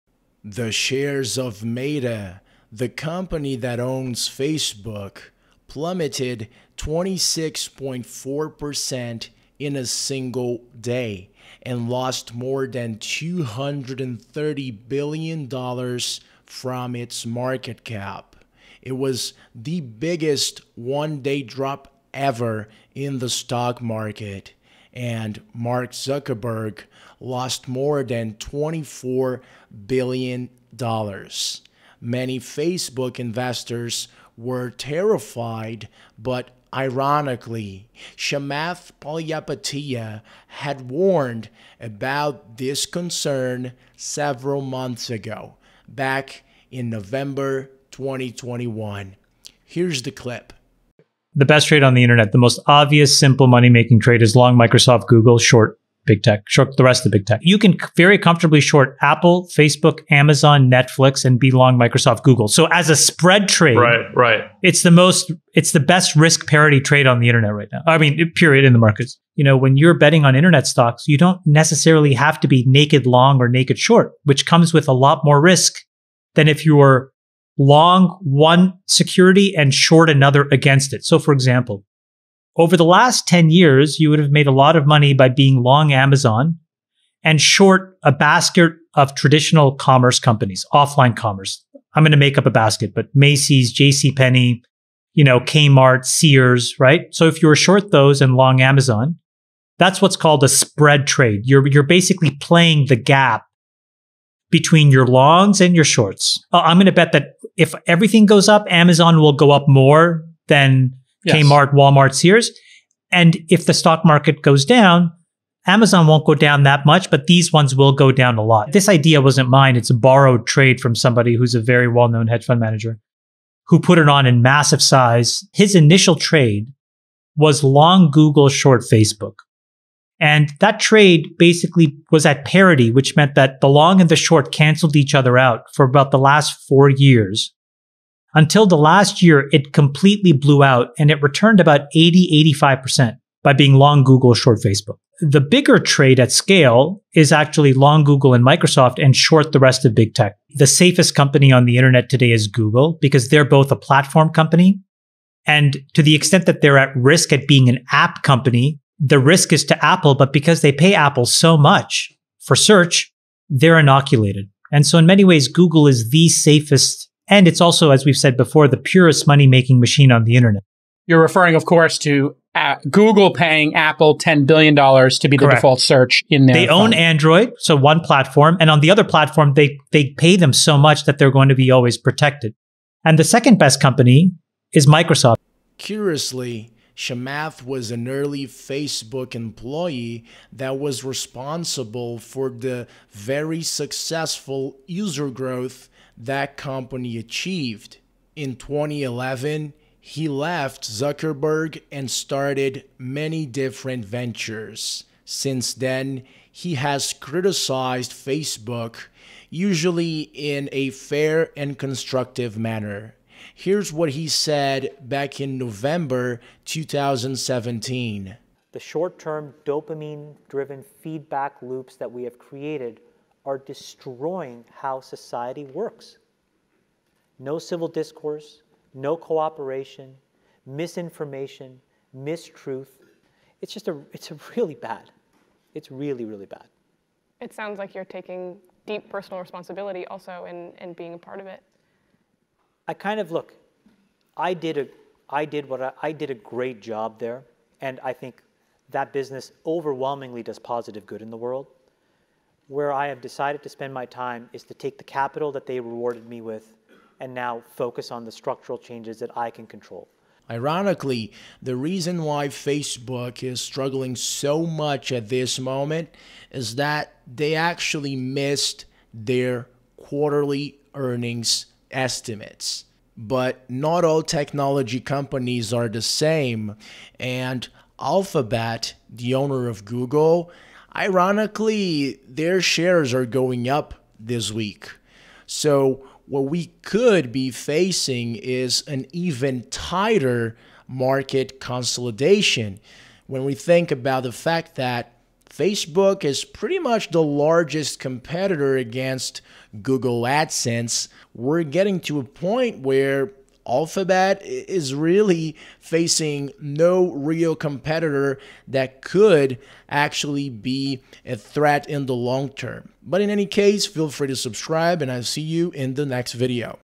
Chamath Palihapitiya Predicted Facebook Stock Crash But No One Listened
Meta stock had the biggest drop in the history of the stock market. Many investors were terrified but Chamath Palihapitiya, the legendary venture capitalist, predicted several months ago that this Facebook stock crash would come.